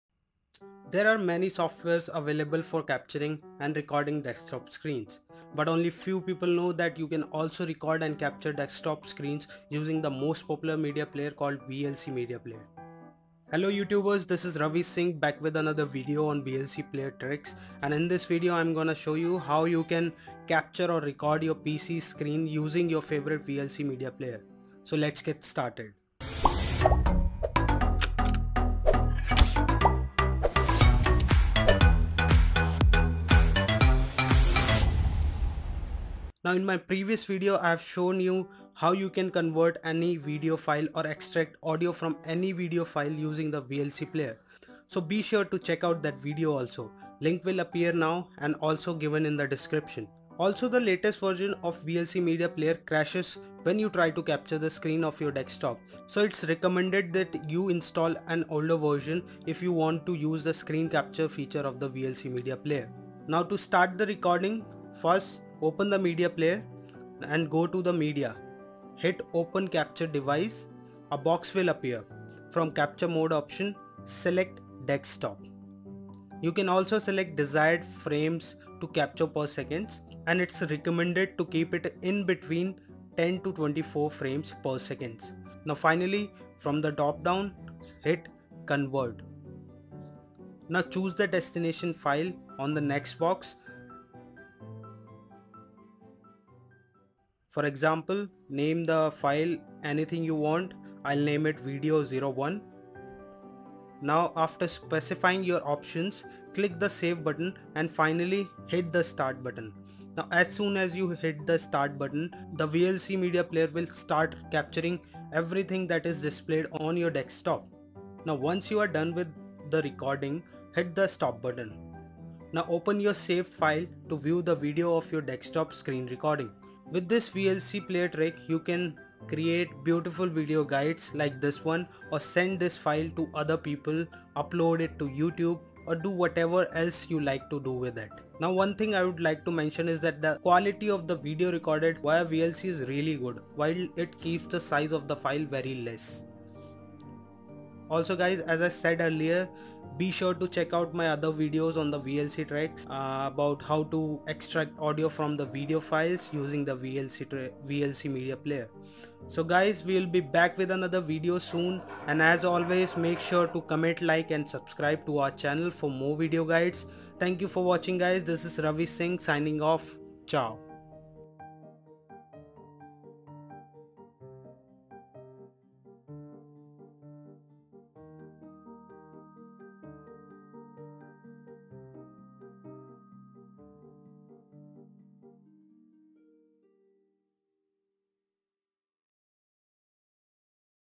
Record PC, Mac, or Linux Screen with VLC Media Player
You can record your Mac, linux or Windows computer desktop screen using the VLC Media Player app. It's really simple and size of captured video is also very less. Most of us are not aware of is, the standalone media player can be also used to capture the screen of computer or create screencasts using VLC Player. Now that you know about this interesting feature in VLC media player.
for More How To Tech Guides and Tutorials.

for More Tech How to guides.